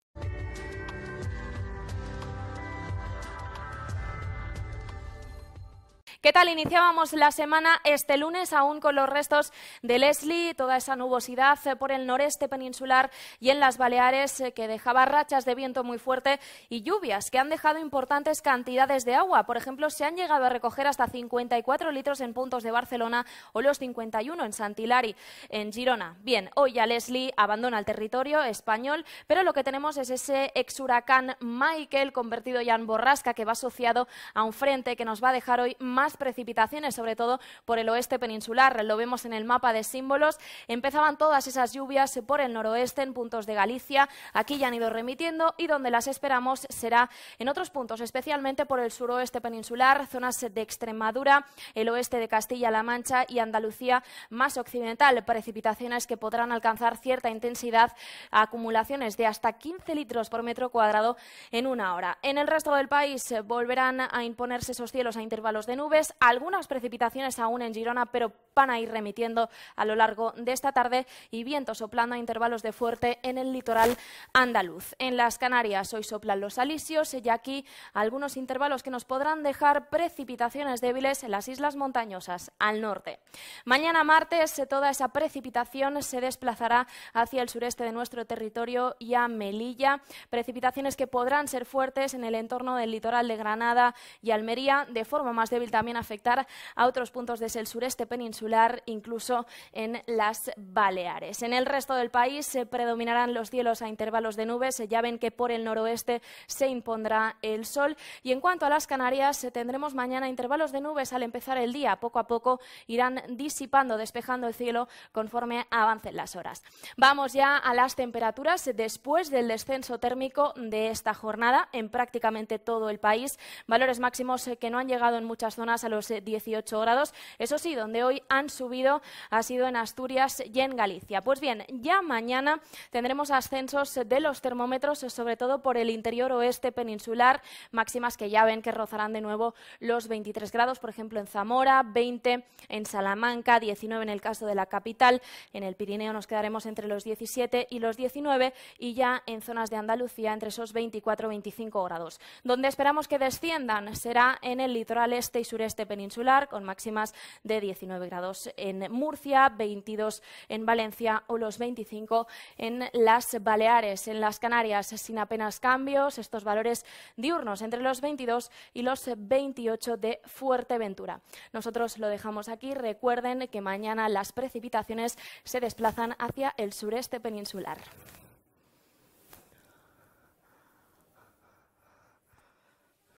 El tiempo de AEMET 15-10-2018
Lunes, 15 octubre 2018
Se esperan precipitaciones en el oeste peninsular, mas frecuentes en el litoral oeste de Galicia, remitiendo por el noroeste y extendiéndose después a Extremadura, oeste de Castilla-La Mancha y Andalucía, donde pueden ser localmente fuertes. En Canarias, probables lluvias débiles en el norte de las islas montañosas. Poco nuboso o con intervalos nubosos en el resto del país.
Martes, 16 octubre 2018
Fenómenos significativos
Posibilidad de chubascos localmente fuertes a primeras horas en los litorales de Granada y Almería.